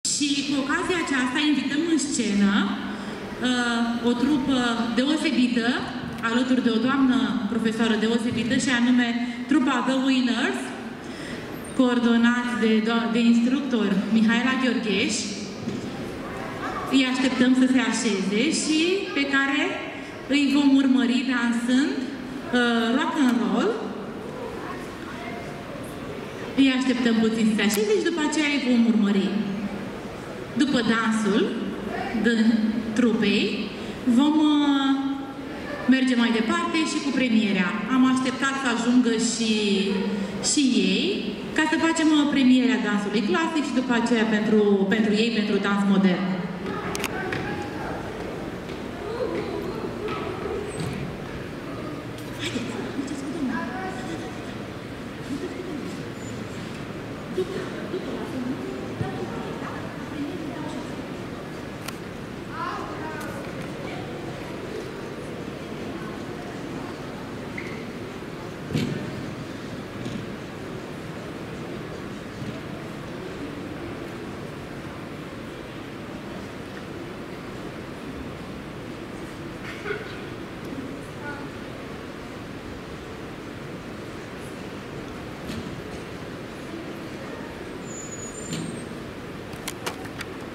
Sarbatoare Artelor Moderne 2024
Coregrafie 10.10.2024
Teatru de Vara Bacău - Jud. Bacău
Romania